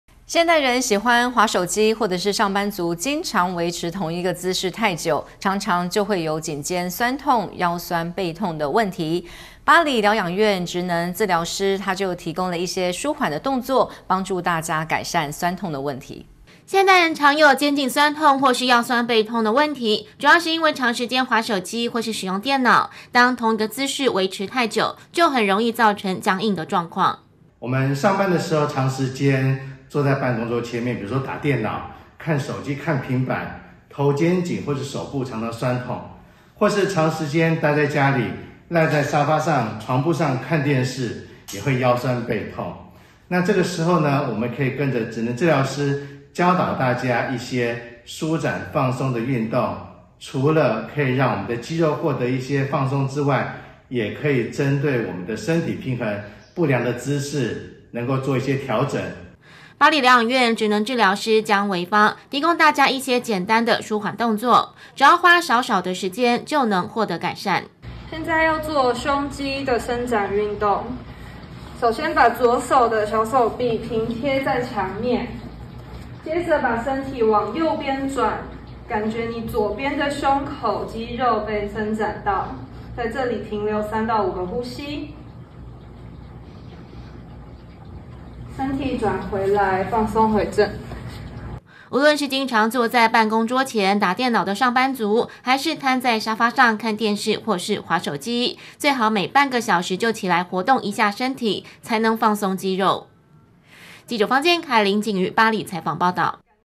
天外天新聞1110223 11長時間打電腦 是否肩頸卡卡
現代人喜歡划手機，或是上班族經常維持同一個姿勢太久，常會有肩頸痠痛、腰痠背痛的問題，八里療養院職能治療師提供一些紓緩動作，幫助大家改善痠痛的問題。